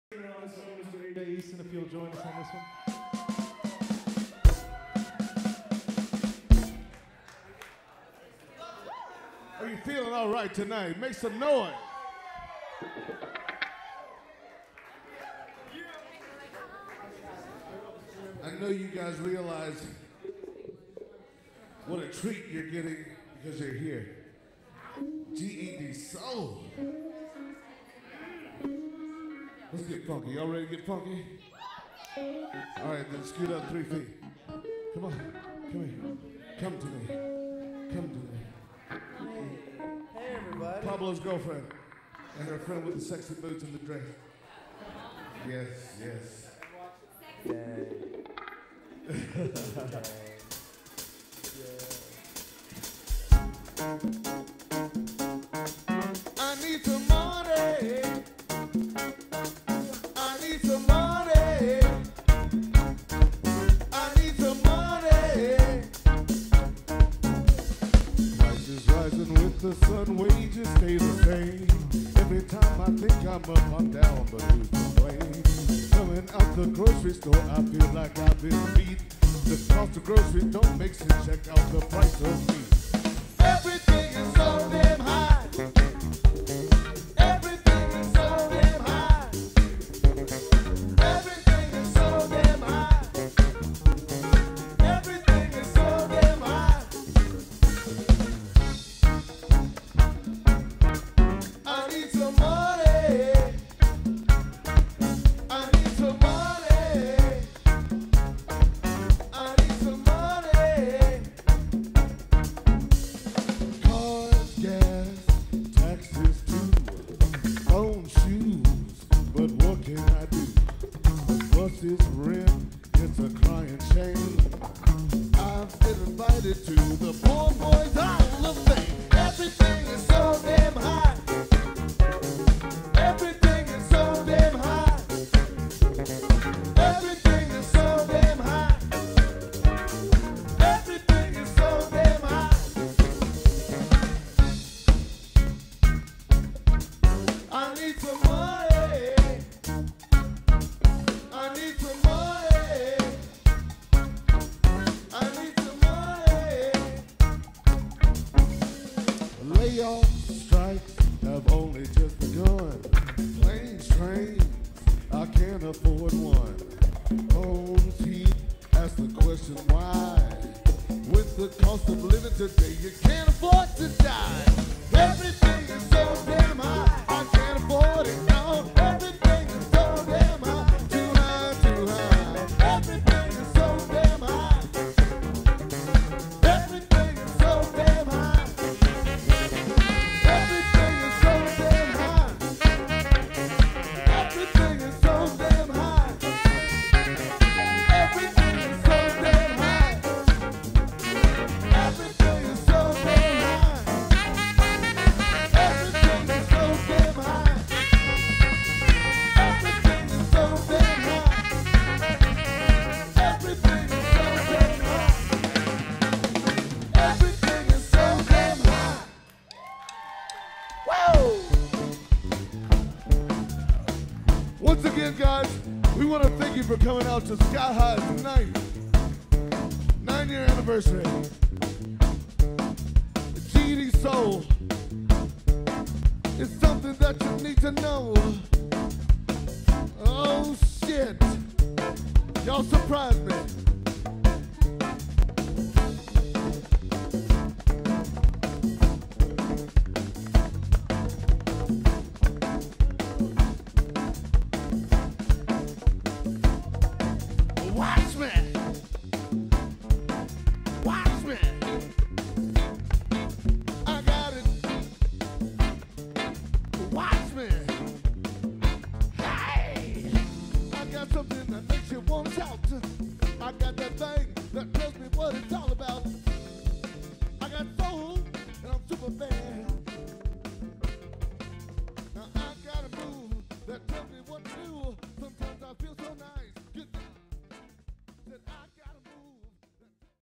Coolin' System w/ AJ Eason Live (HQ)
Coolin' System perform live with special guest AJ Eason.